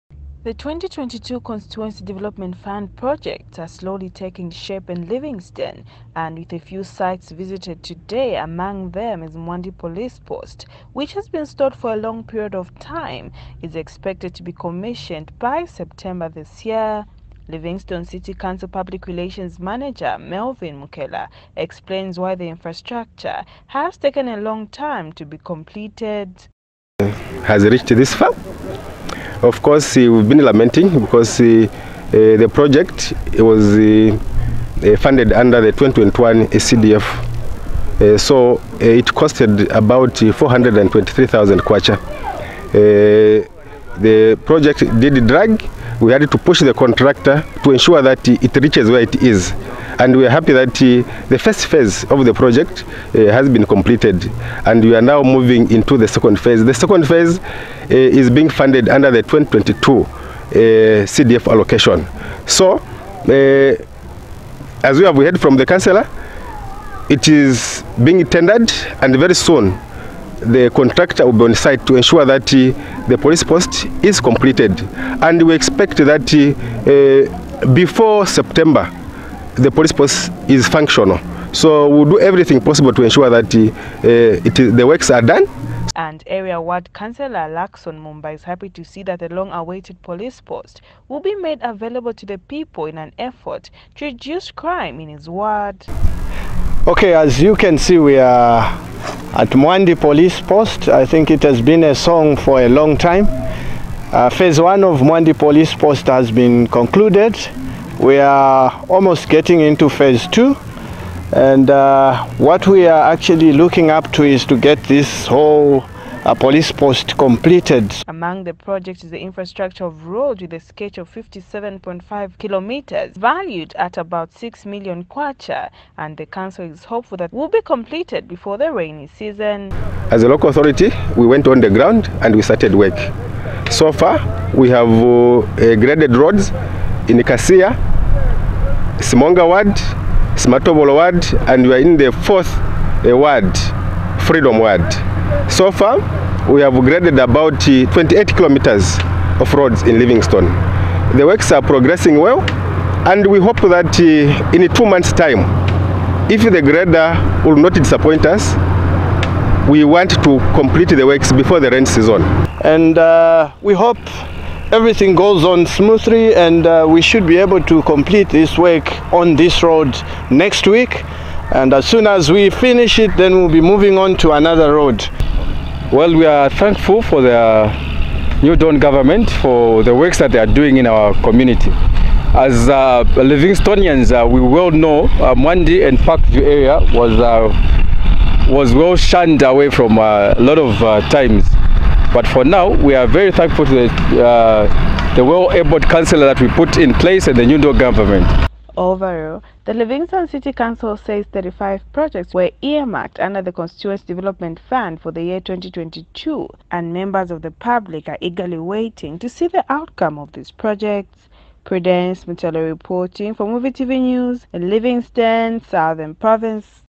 LIVINGSTONE CDF REPORT.
The Livingstone City Council says most of the projects that were funded under the 2022 Constituency Development Fund are near completion.